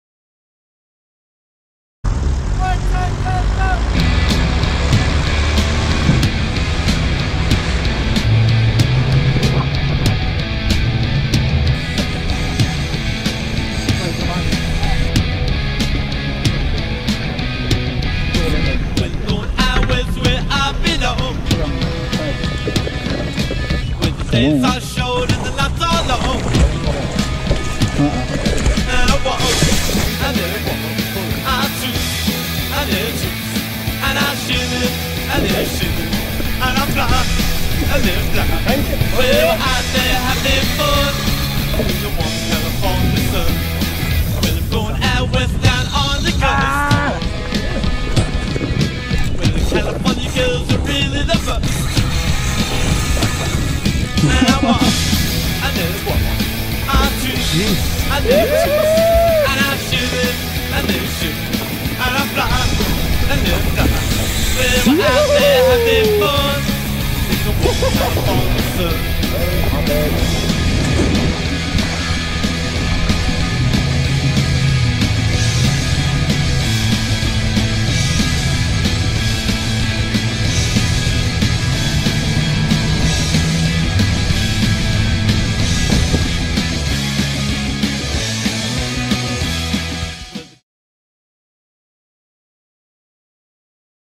Ballito Offshore Fishing, South Africa | Yellowtail, Dorado, Bonito
Barend and his dad went on an offshore trip from Ballito on their boat "CHAOS".  He managed to catch a massive yellowtail, 4 Dorado and a bonito.  The yellowtail went on a few runs and took some line before he managed to haul it in.  Successful day on the sea to say the least.